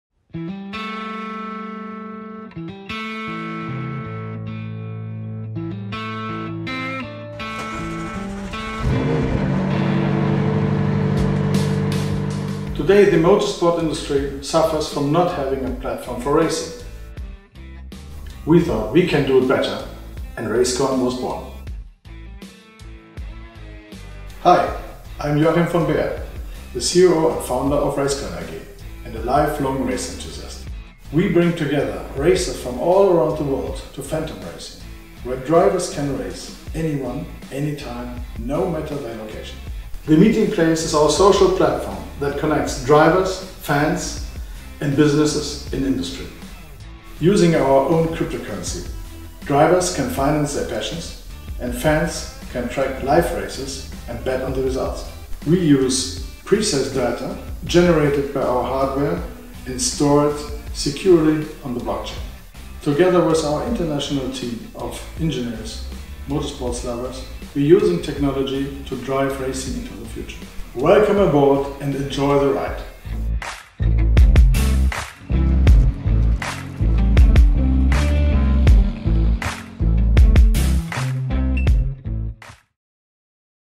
How Racecoin was born - A message from the CEO
Today, the motorsport industry suffers from not having a platform for racing. We thought we can do it better and race-coin.io was born. Watch our first video by Joachim von Behr explaining what race-coin.io does and how we will revolutionize the racing world. Do you want to join the ride?